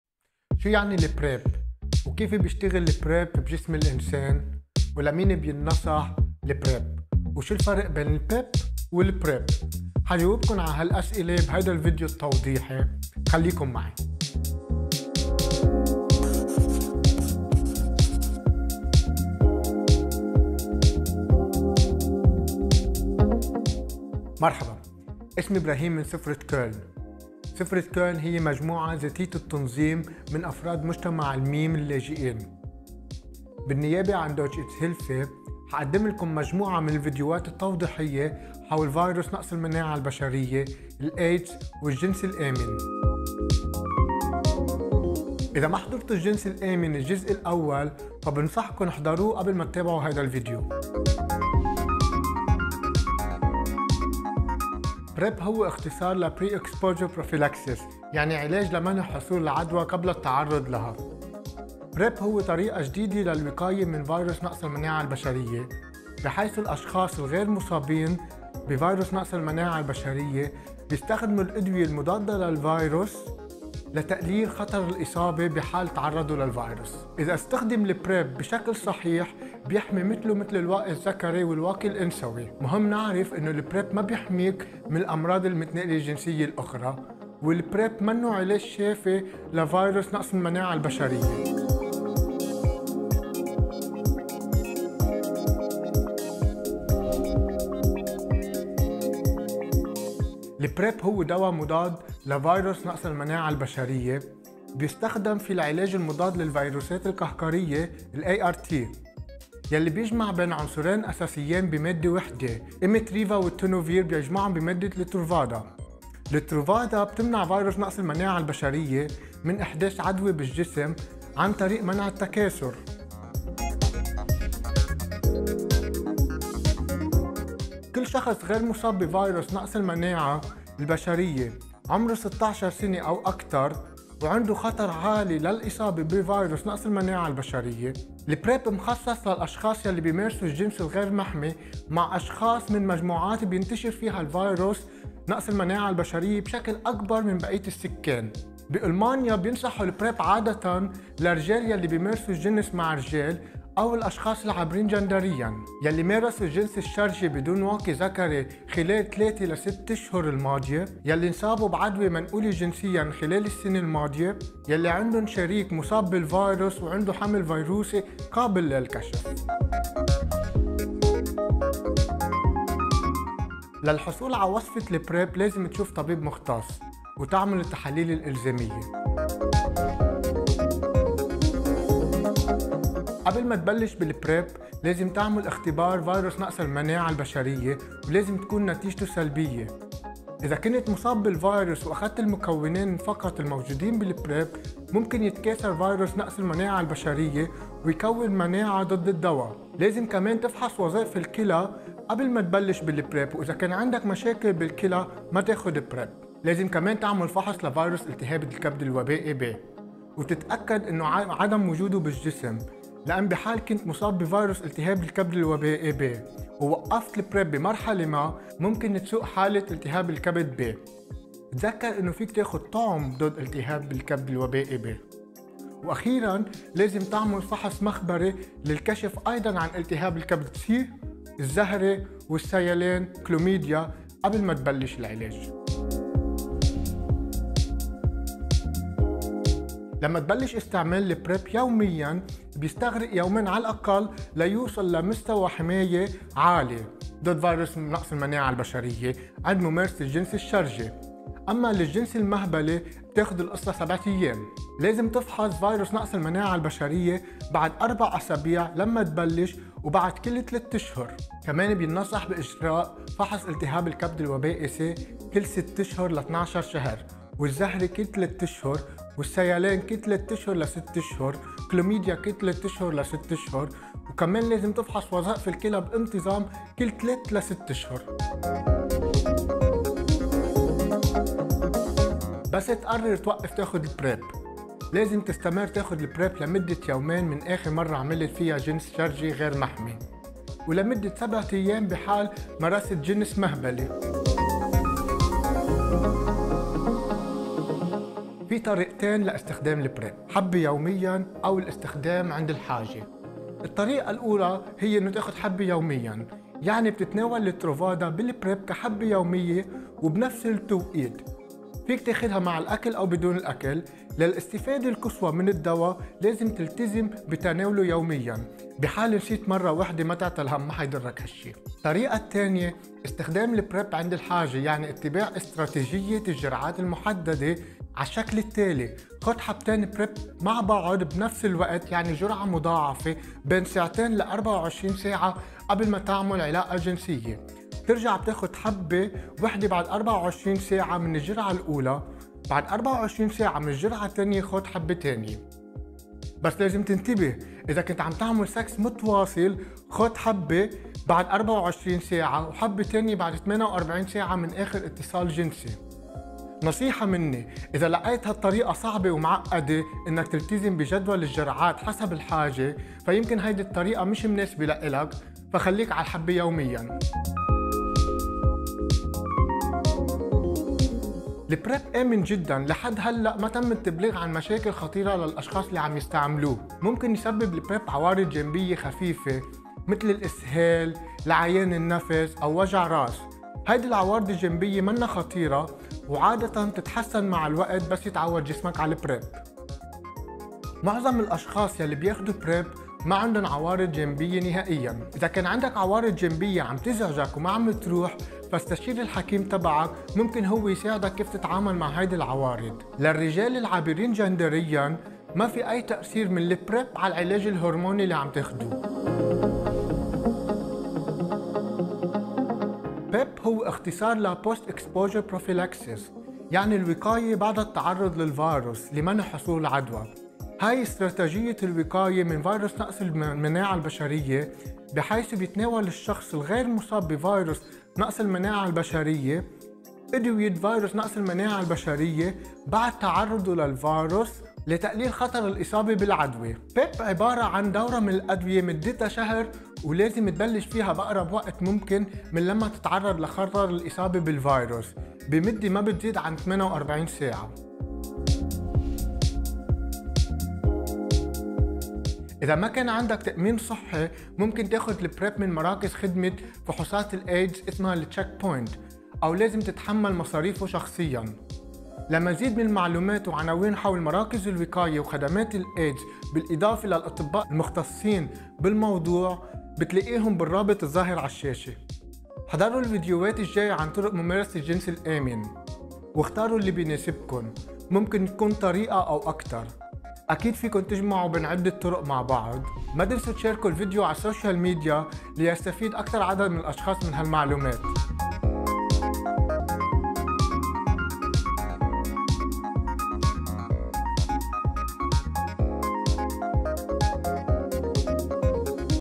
البريب (الوقاية من التعرض المسبق) - مقاطع فيديو متعددة اللغات حول الجنس الآمن للاجئين المثليين
توفر مقاطع الفيديو معلومات حول الجنس الآمن (الواقي الذكري ، البريب, العلاج الوقائي) وتشرح الوسائل للوصول إلى العلاج في ألمانيا.
وهي موجهة للاجئين المثليين وهي متوفرة باللغات التالية: الإنجليزية والعربية.
تم إنشاء مقاطع الفيديو بواسطة إبراهيم فيليكي (سفرة كولن) بالنيابة عن المنظمة الألمانية لمساعدة الايدزوبالتعاون معها.